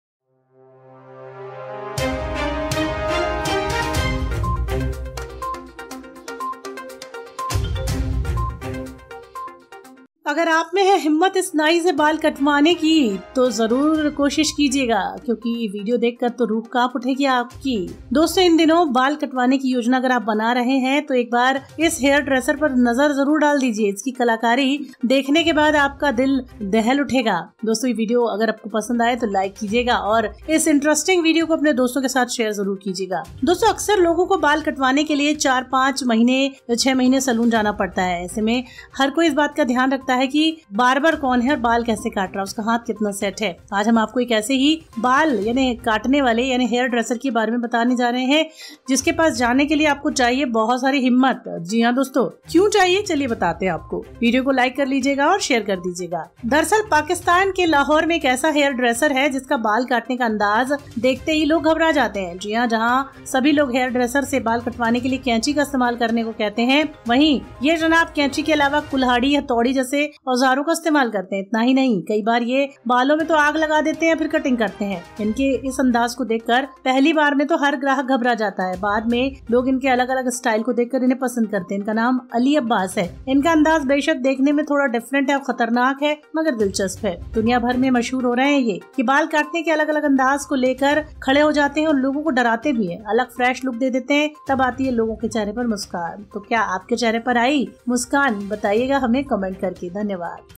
इस तरीके से सभी का बाल काटता था यह नाई, जब सच्चाई आई सामने तो सबके होश उड़ गए
Often people have to go to the salon every 4-6 months to get a haircut. In such a situation, everyone definitely cares about who cuts the barber and whose hand is set so well. However, today we are going to tell you about such a hair-cutting person, who first of all needs a lot of courage to go to you.

HJ NEWS !!